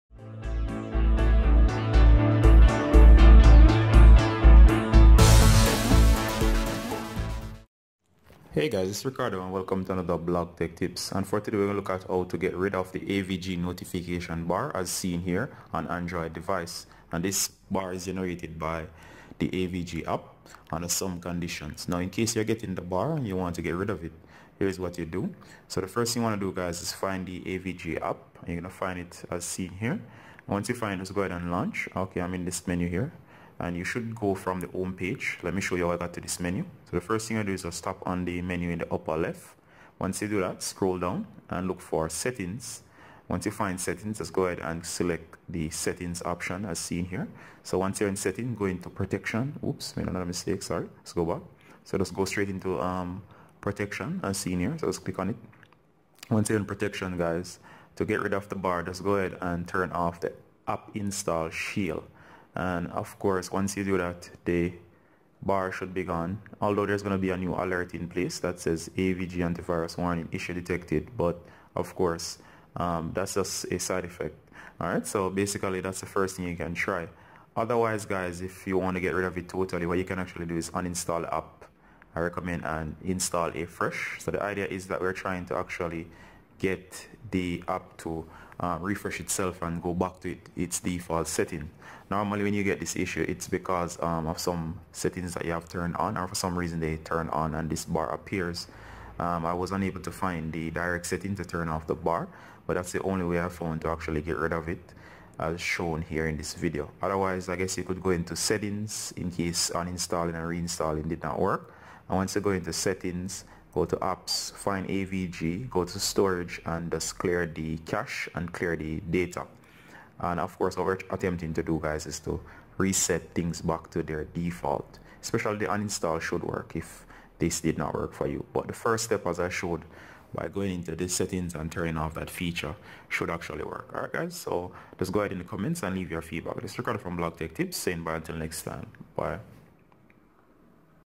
Get Rid of AVG Notification Bar on Android Fix
This is how you can Get Rid of AVG Notification Bar on Android with the four icons that's set in the Notification area of your phone. Its really annoying especially if you have no idea how it was turned on and what to do to remove it.